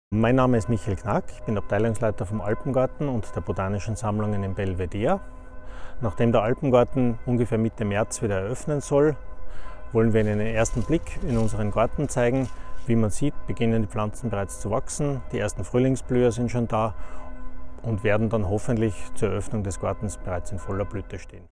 Alpengarten im Belvedere
Die Alpengarten Saison 2019 beginnt am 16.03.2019, Schau vorbei und bestaune die Flora der Alpen mitten in Wien!